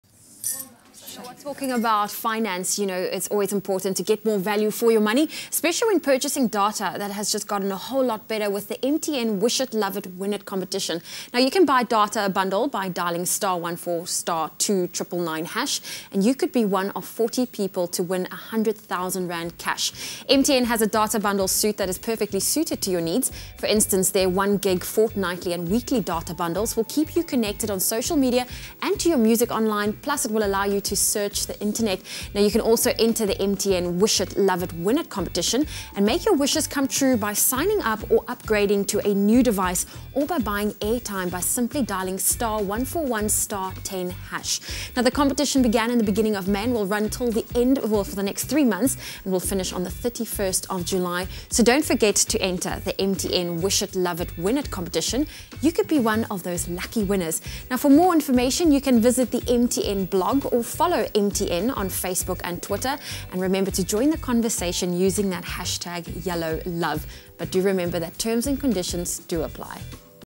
Win R100 000 with MTN’s Wish It, Love It, Win It
Getting more value for money when purchasing data has just gotten a whole lot better with the MTN Wish It, Love It, Win It competition. Buy a data bundle by dialling *141*2*999# and you could be 1 of 40 people to win R100 000 cash. Ts&Cs apply.